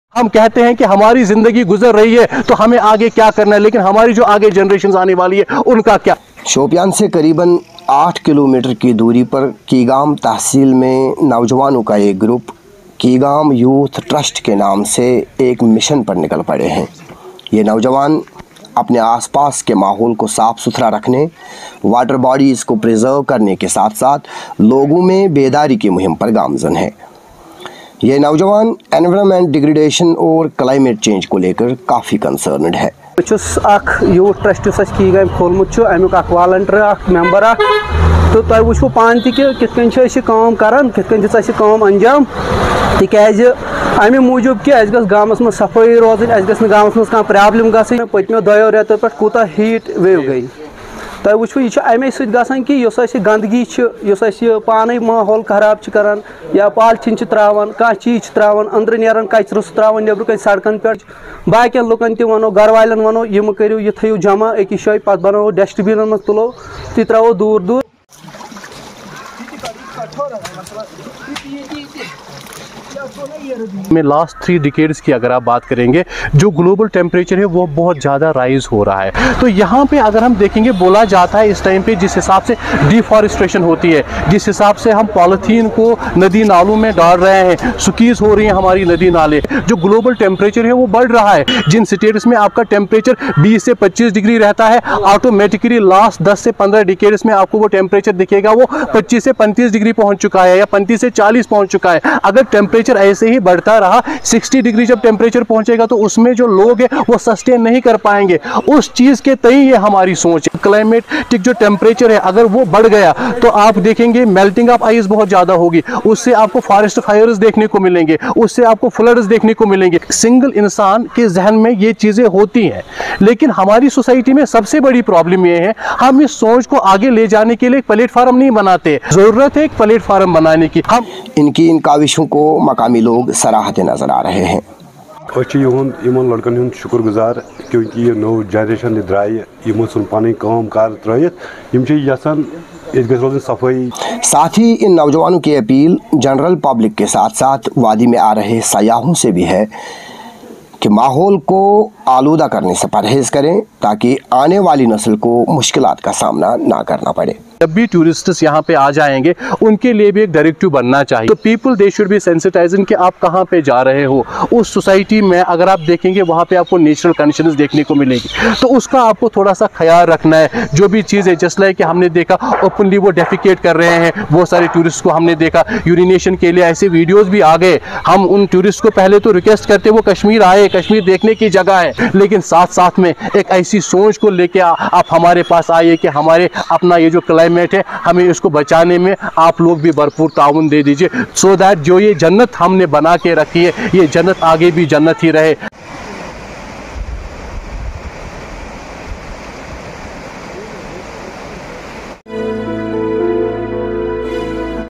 Youth-Led Initiative in Shopian: Combatting Environmental Degradation and Climate Change
In Tehsil Keegam, Shopian district, a group of proactive youth is taking a stand against environmental degradation and climate change. Concerned about the deteriorating state of their surroundings, they have launched a community-driven initiative to clean their locality. This involves clearing lanes and water bodies and raising awareness about the harmful effects of polythene use and deforestation.

Their efforts have garnered widespread applause from the local community, inspiring many to join their cause. The group actively appeals to the general public and tourists, urging them to minimize polythene usage and utilize proper washroom facilities. This video showcases the commendable work of these young environmentalists, highlighting their dedication to preserving the natural beauty of their region and promoting sustainable practices. Through interviews and on-the-ground footage, we capture the spirit of community collaboration and the positive impact of grassroots activism on environmental conservation.

This story is by Basharat Amin, a Community, Content Creator from Shopian, Kashmir. Learn more about Basharat and see other videos of his/hers